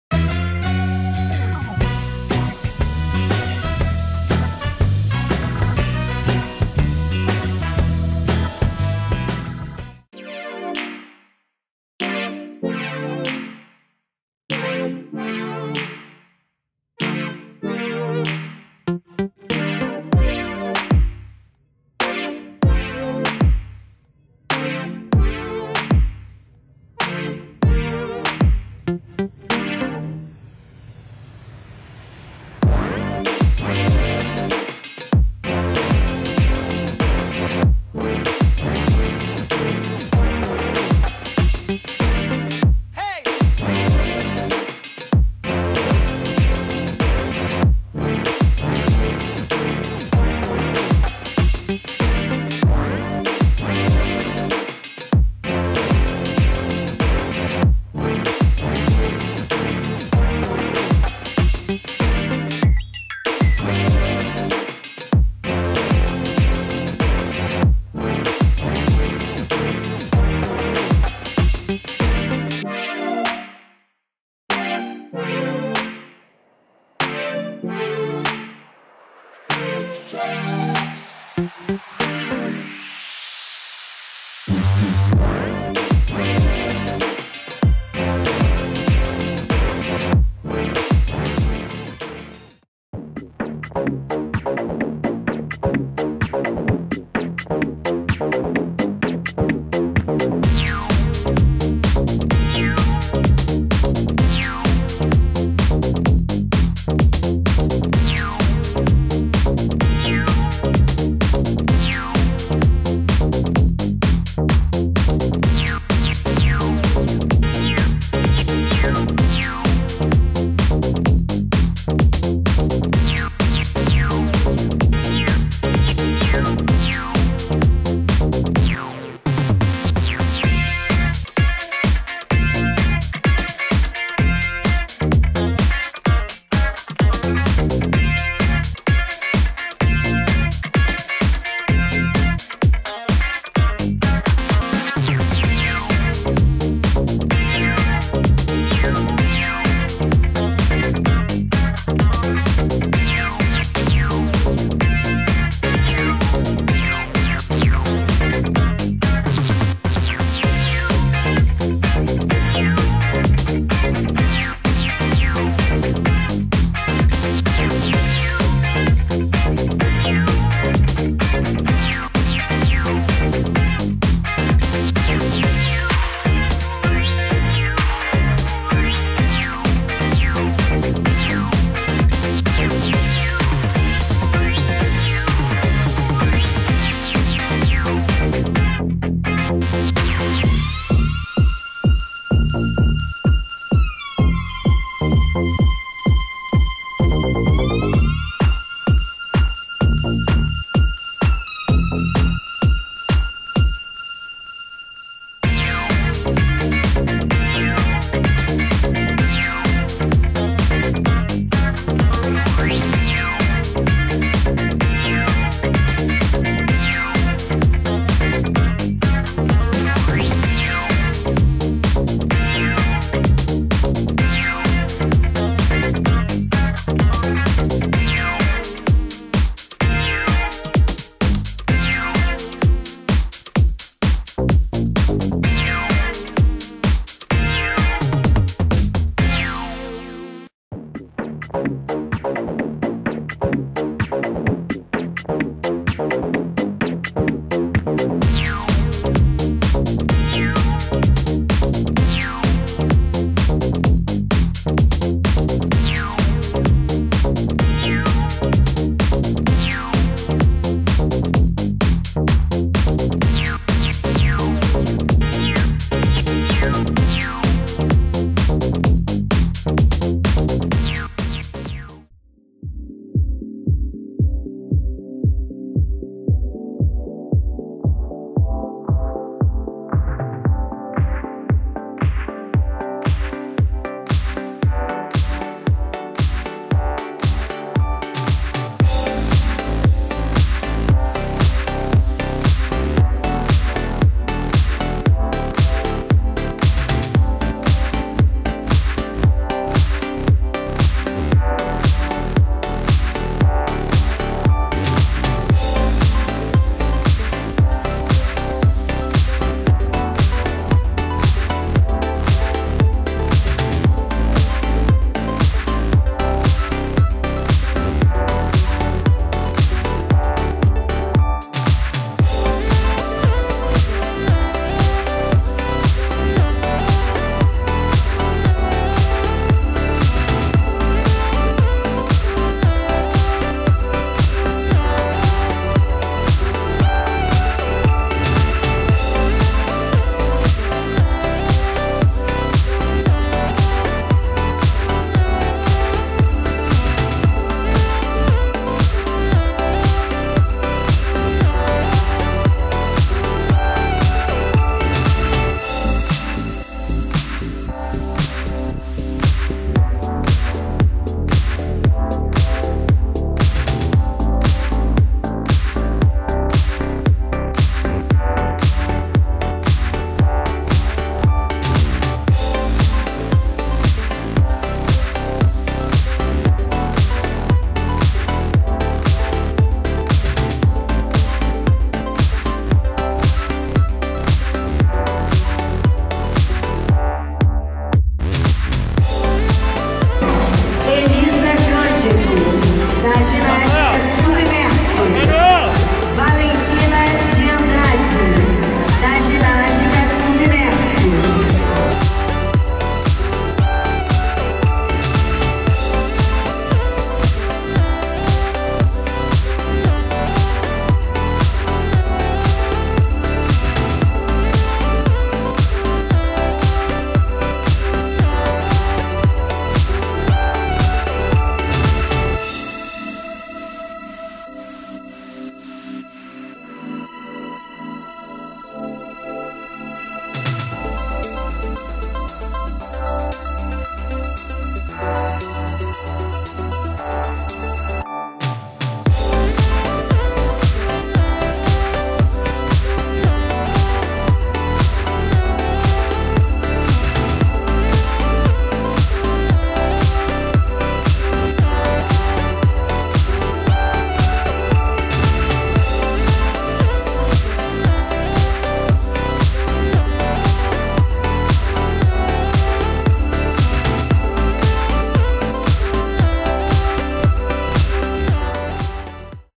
ESFORÇO E DEDICAÇÃO! Campeonato estadual de ginástica artística-2019
Oi, pessoal!
Chegou mais um campeonato estadual e aqui estamos nós mais uma vez.
Treinamos bastante e agora é hora de dar o nosso melhor. É claro que bate aquele vervosismo na hora da apresentação em cada aparelho. Rsrsrs... Mas é normal.

Acompanha o vídeo aí e não se esqueça de dar o seu like!

E lembre-se:
 O ESFORÇO DE CADA DIA TE FAZ TER UM AMANHÃ MELHOR.
DEDIQUE-SE!

Beijos
Las Veigas**